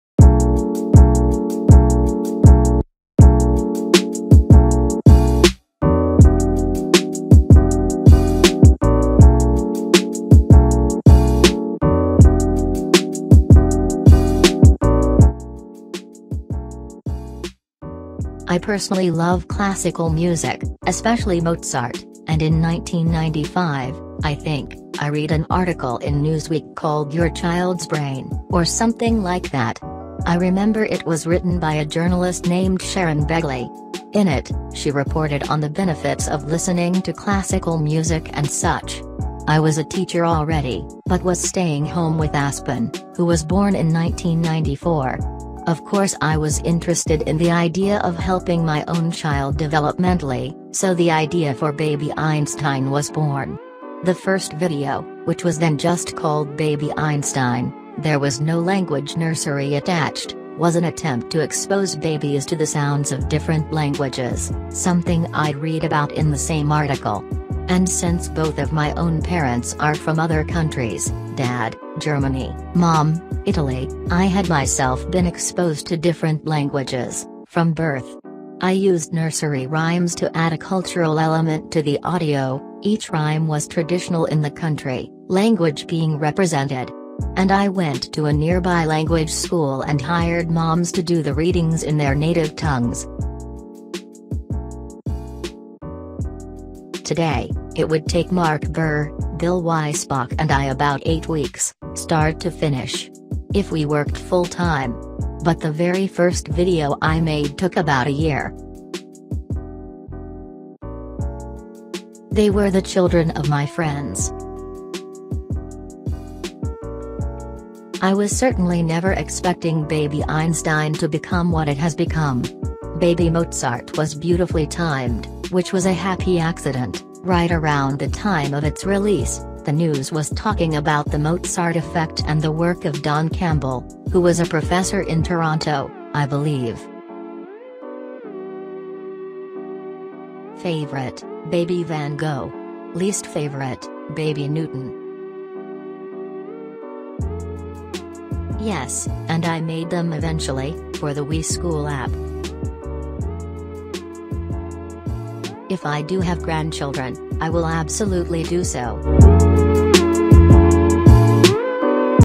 I interviewed Julie Clark Interview (TBEGP Archival Footage)
Yes you heard me right... I interviewed Julie Clark once.

Originally archived from: December 2020